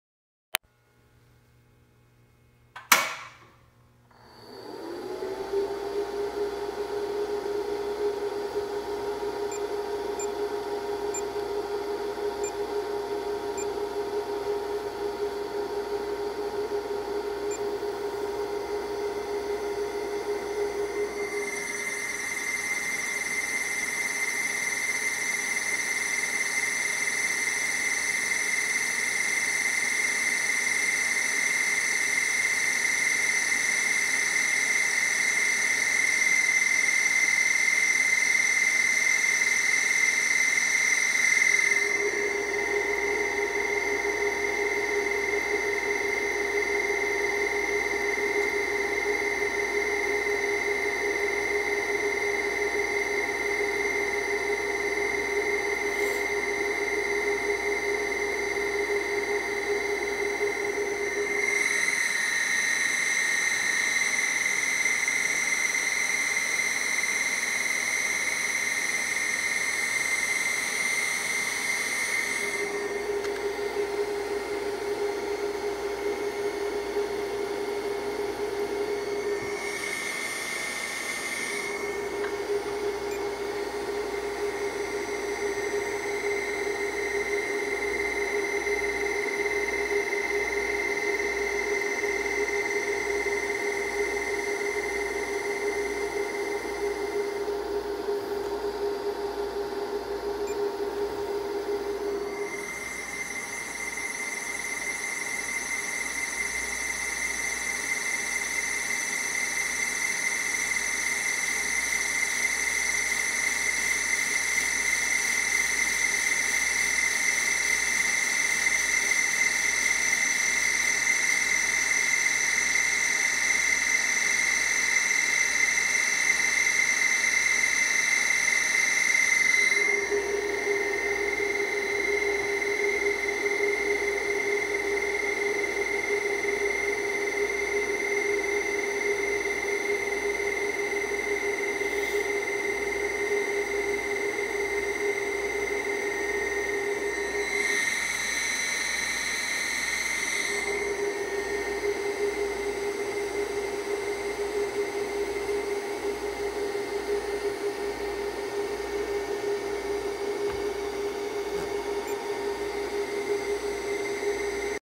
405801-1 - MGI Technologies Drive - M4500-040 - M54497009
Testing on 1HP motor with 60 sec. ramp up time